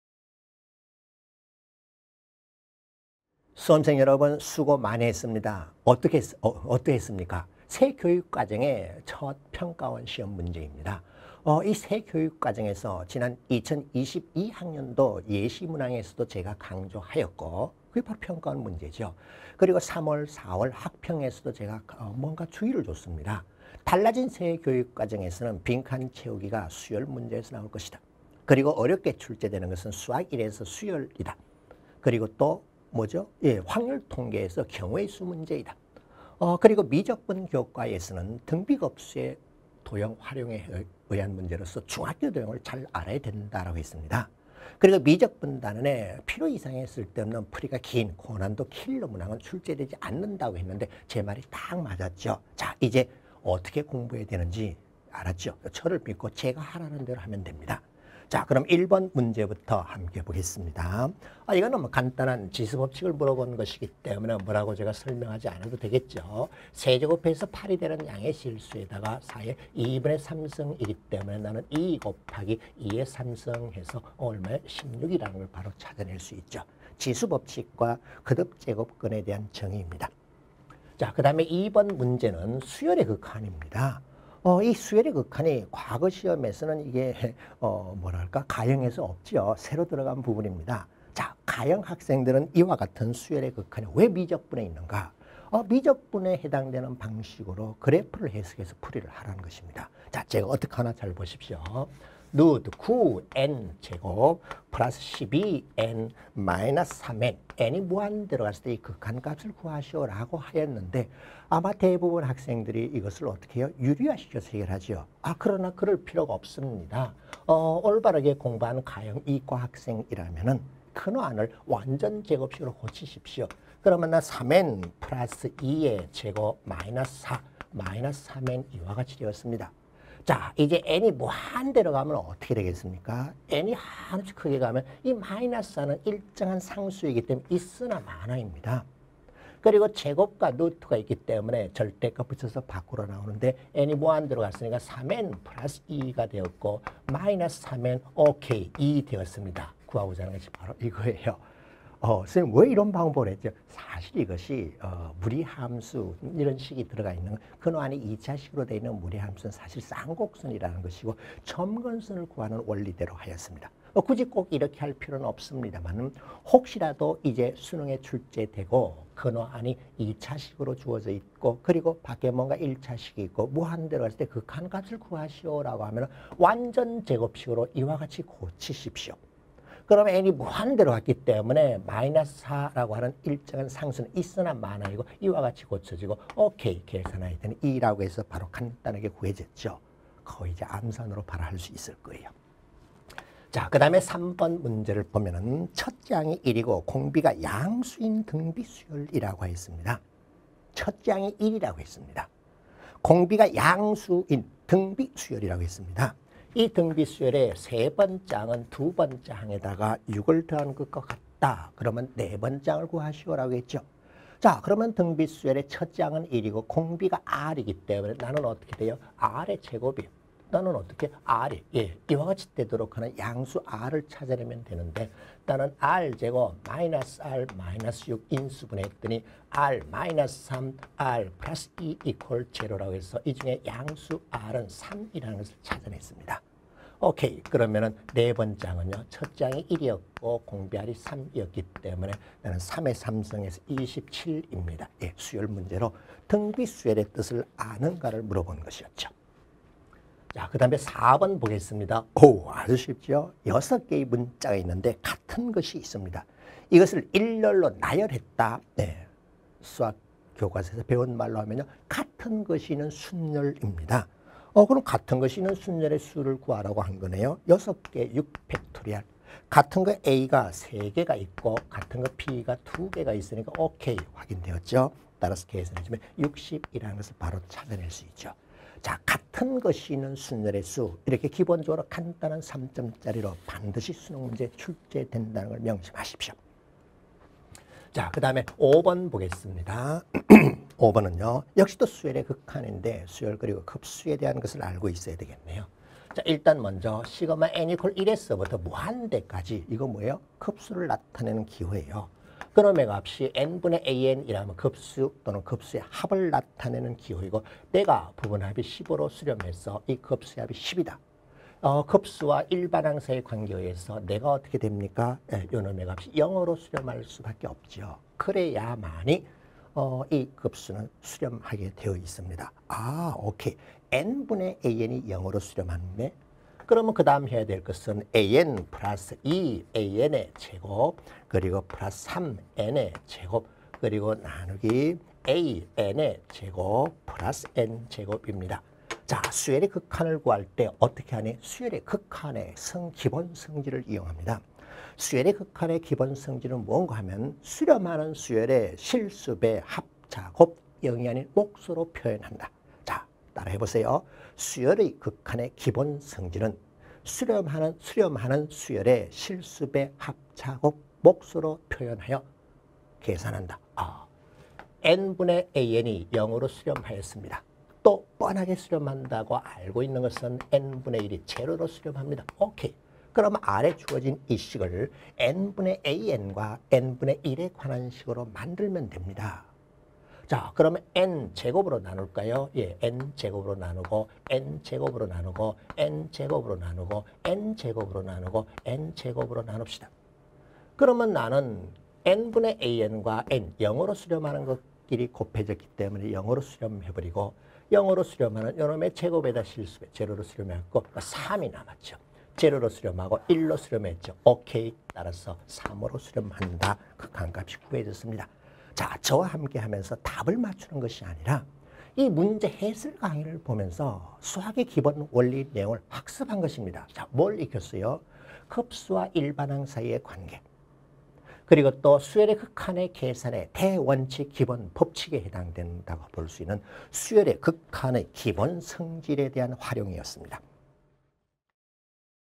[메가스터디] 수학 박승동 쌤 - 고3 6월 모평 수학 가형 해설강의
6월 모평 고3 수학 가형 해설강의!
박승동 선생님의 해설강의를 만나보세요!

박승동 선생님에 대해 더 알고싶다면?